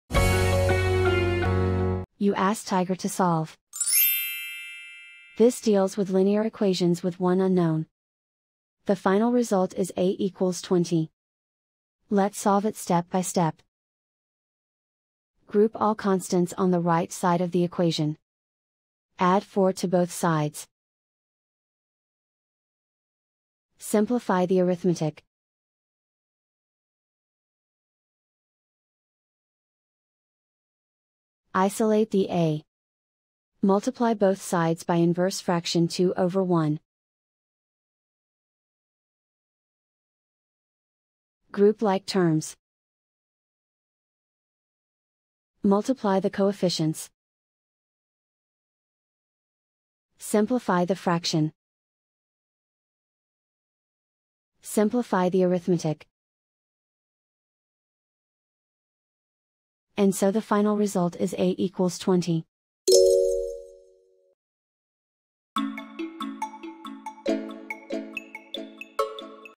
Linear equation with one unknown: Solve 1/2a-4=6 step-by-step solution
Solving 1/2a-4=6 using the linear equation with one unknown solver

for a step-by-step and more tips and tricks for dealing with linear equations with one unknown.